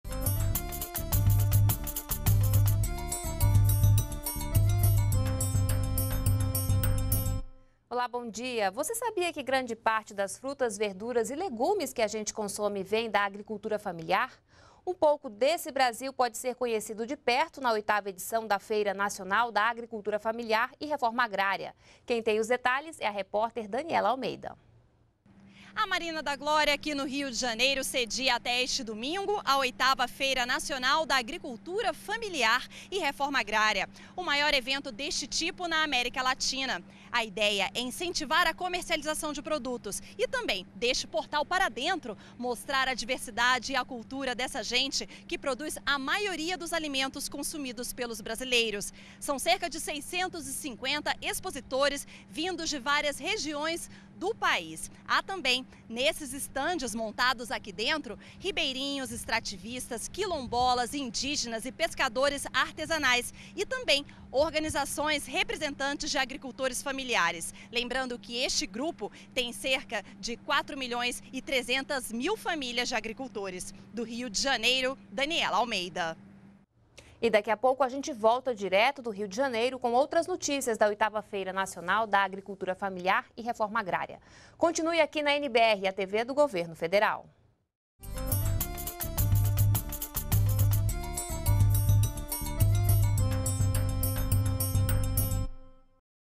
Maior evento de agricultura familiar da América Latina acontece até domingo no Rio de Janeiro
FEIRA MDA - 22.11.12: Um pouco desse Brasil pode ser conhecido de perto na 8ª edição da  Feira Nacional da Agricultura Familiar e Reforma Agrária.Você sabia que grande parte das frutas, verduras e legumes que a gente consome vem da agricultura familiar? A repórter Daniella Almeida traz os detalhes da maior Feira deste tipo na América Latina.